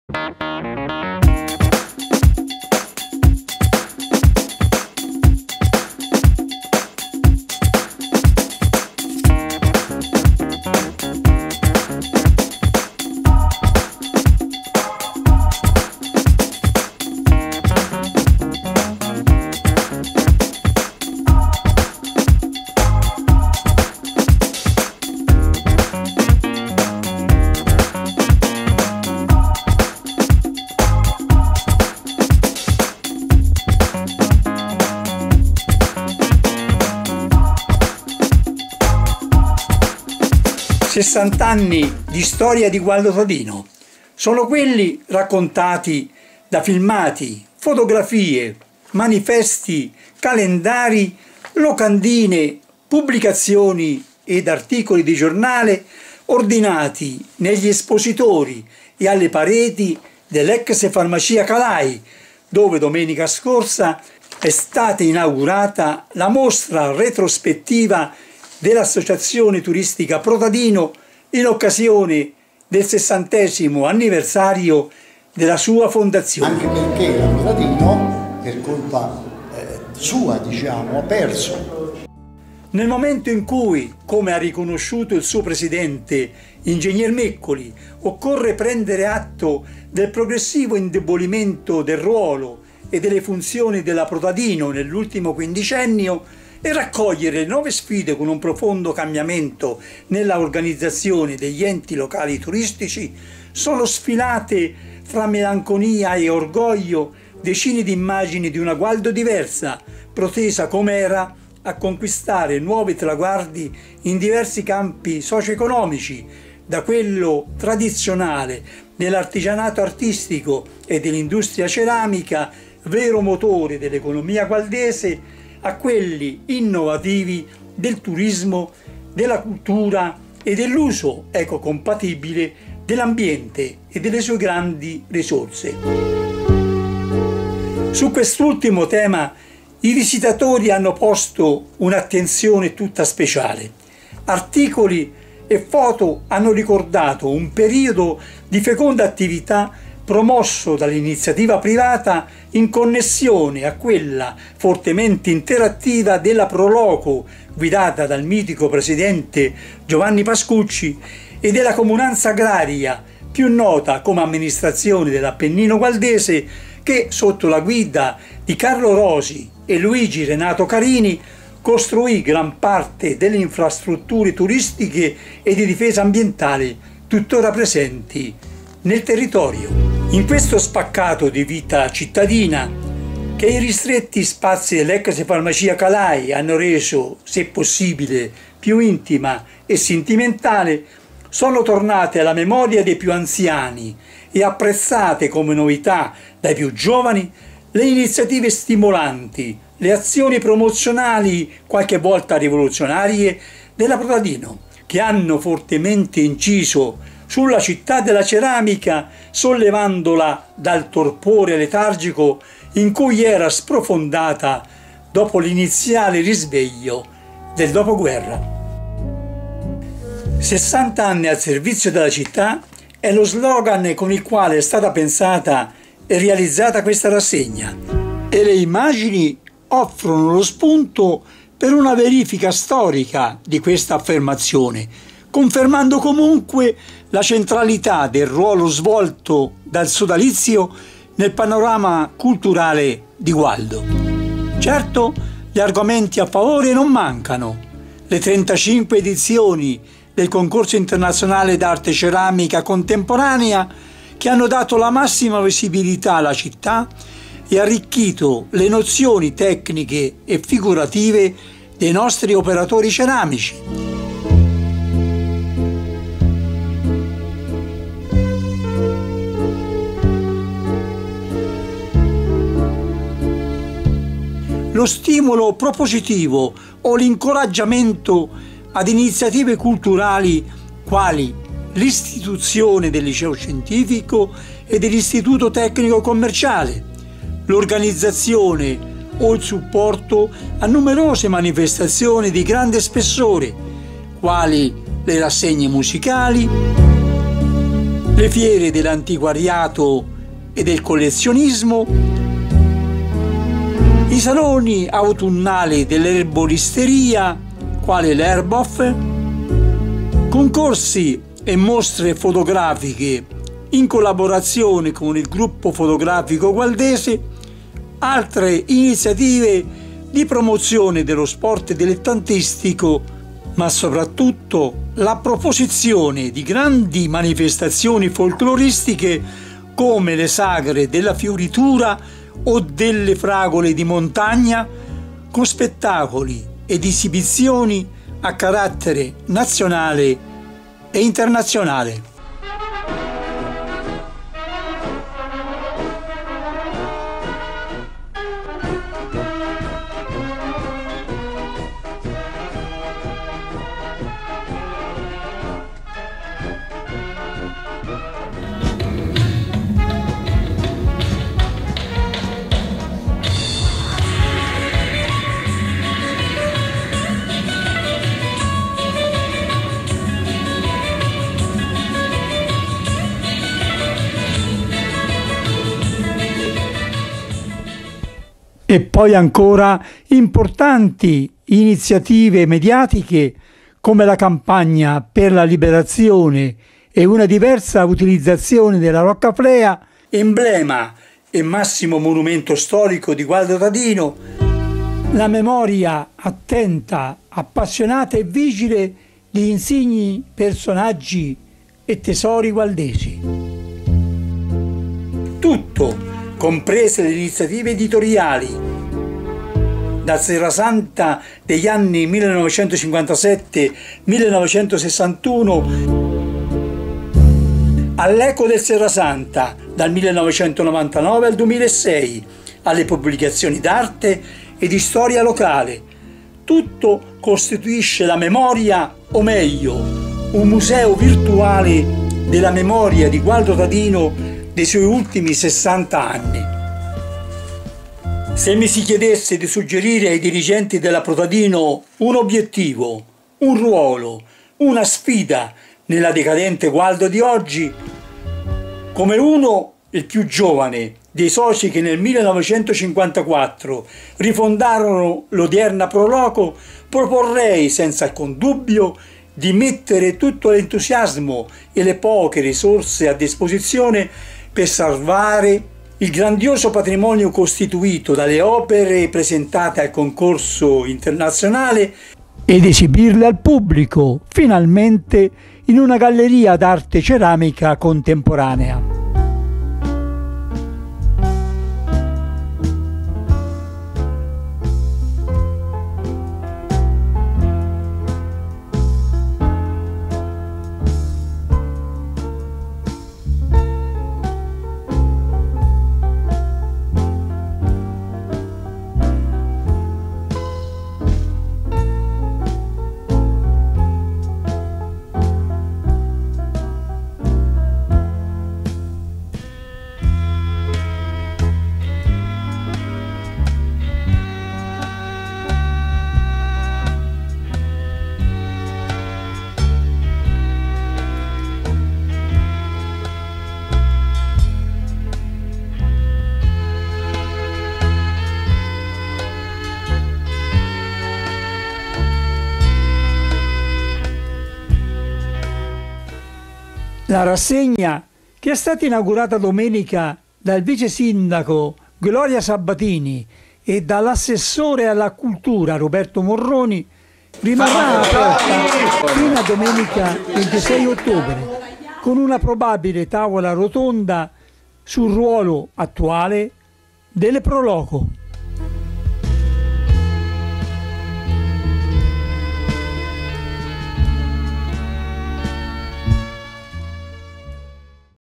60 Pro Tadino
HA UN FUTURO LA SESSANTENNE ASSOCIAZIONE PRO TADINO?
Gualdo Tadino.- Inaugurata la mostra retrospettiva che l'Associazione turistica Pro Tadino ha allestito per ricordare la sua rifondazione e i "60 anni al servizio della città".
Servizio di Angelo Barberini.